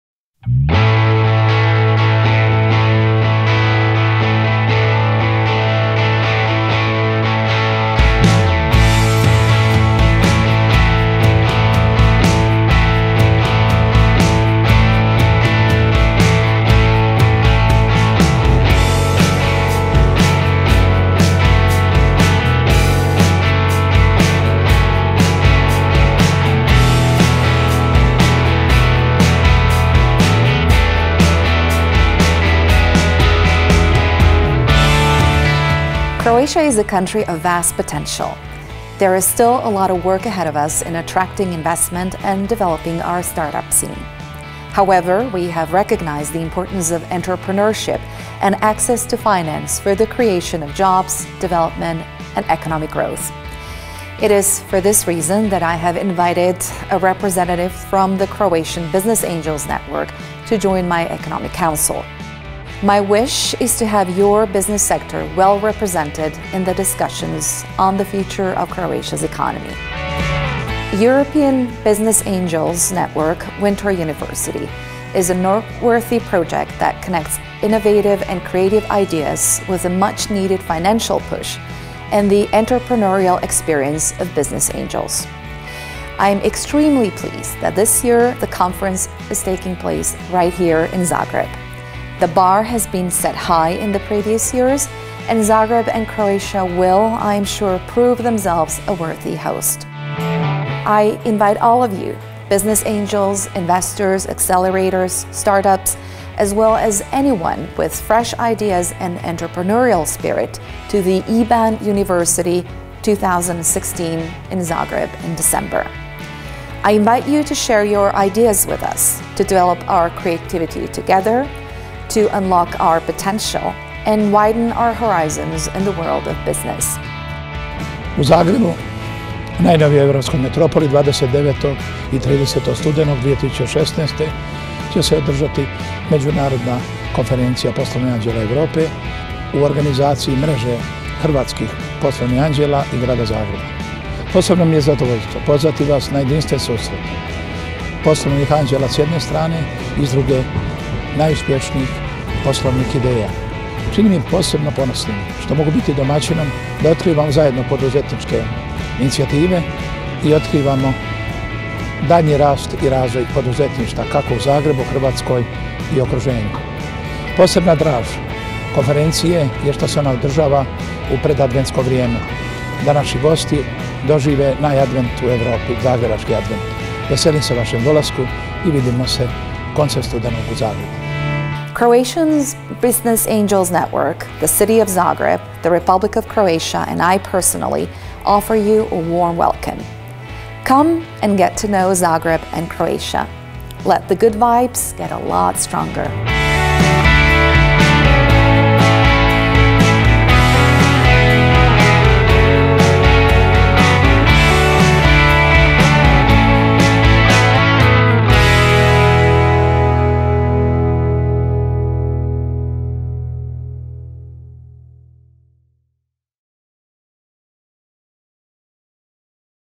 Invitation to EBAN Winter University 2016. Zagreb, Croatia
Invitation to EBAN Winter University 2016. by Croatian President Mrs. Kolinda Grabar - Kitarović and mayor of Zagreb Mr. Milan Bandić. Welcome to Zagreb on 28., 29. and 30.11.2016.